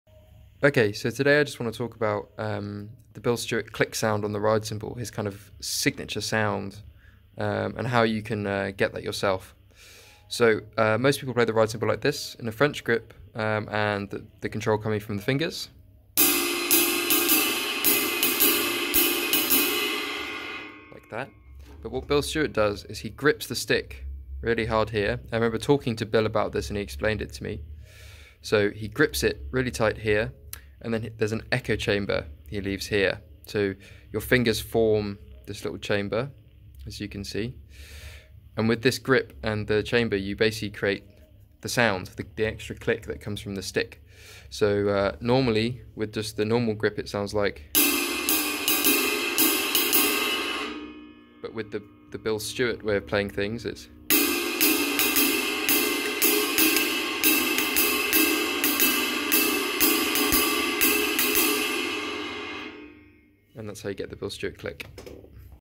How to get Bill Stewart's signature "CLICK" sound on the ride cymbal.
How to play the Bill Stewart "click" sound.

This technique is also often used by Jochen Rueckert and Ramon Montagner.

My phone doesn't do a great job of recording the sound, so best to try this yourself to hear the difference between the techniques.